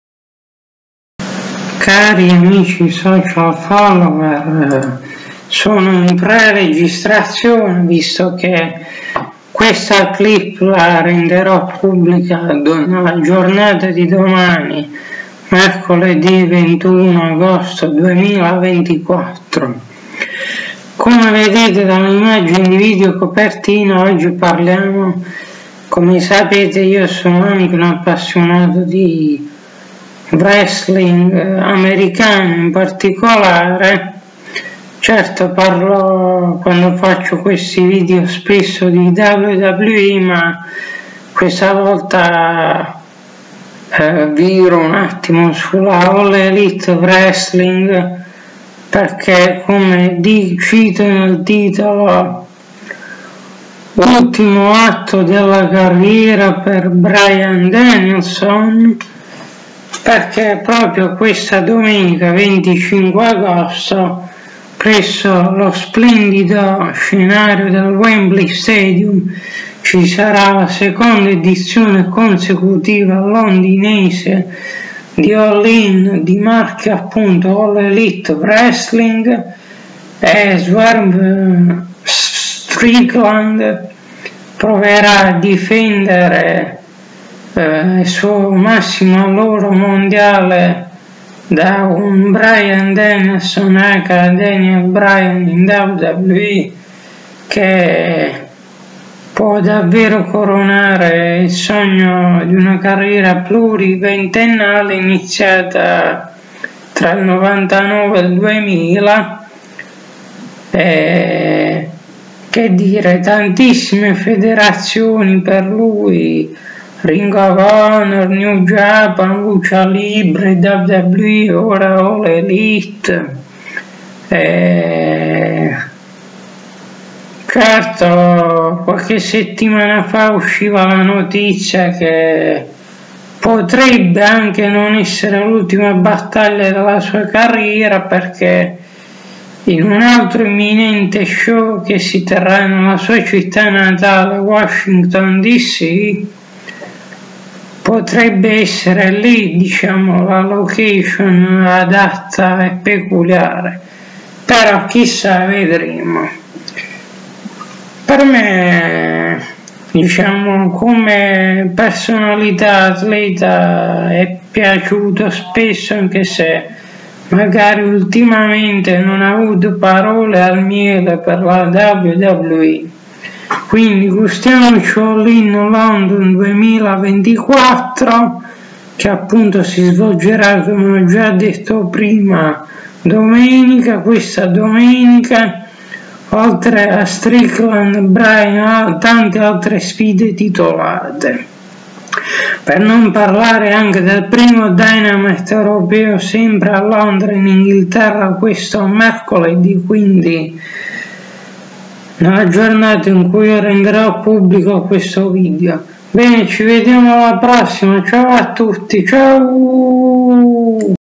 AEW ALL IN LONDON 2024, BRYAN DANIELSON CHIUDERA' DAVVERO LA SUA CARRIERA?
BEN RITROVATI A VOI TUTTI CARI AMICI SOCIAL FOLLOWERS DAL VOSTRO DELUXE. CI SIAMO, E' GIUNTO IL MOMENTO DELLA VERITA': BRYAN DANIELSON (AKA DANIEL BRYAN IN WWE) RIUSCIRA' A CONQUISTARE IL MASSIMO ALLORO MONDIALE DELLA ALL ELITE WRESTLING QUESTA DOMENICA AD ALL IN LONDON WEMBLEY II? FORZA BRYAN, SIAMO TUTTI CON TE!!!